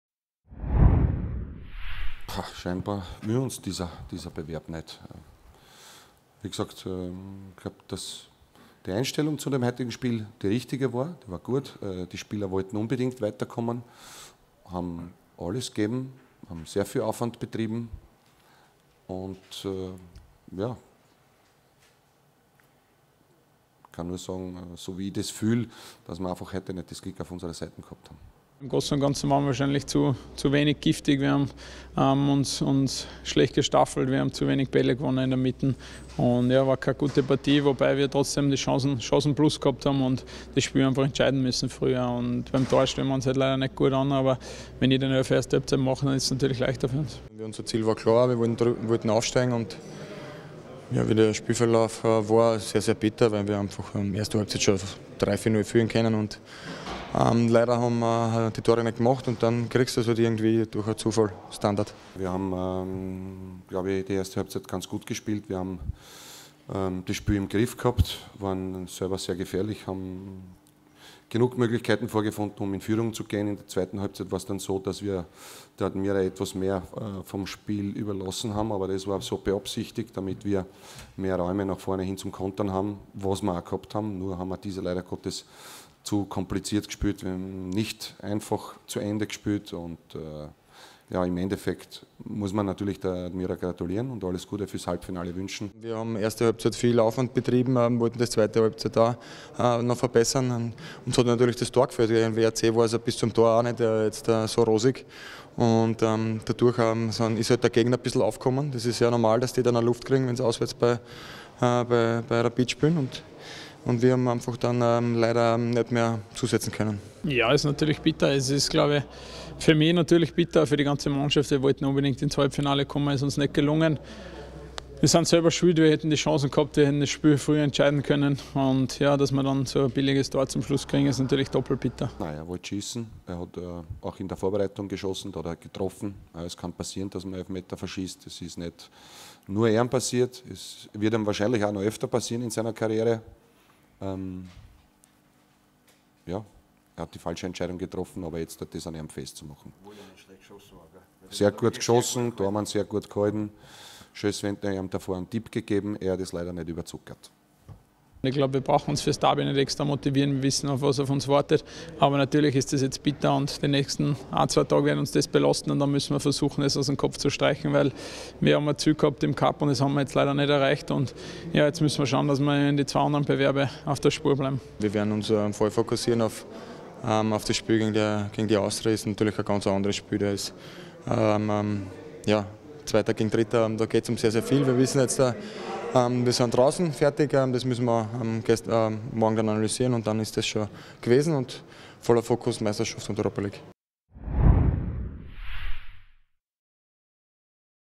Reaktionen nach dem bitteren Cup-Out gegen die Admira
Große Enttäuschung für und bei Grün-Weiß nach dem 0:1 im ÖFB-Samsung-Cup-Viertelfinale gegen die Admira am 10. Februar 2016 im Ernst-Happel-Stadion. Das sagten Zoran Barisic, Stefan Schwab und Mario Sonnleitner nach dem Pokal-Aus!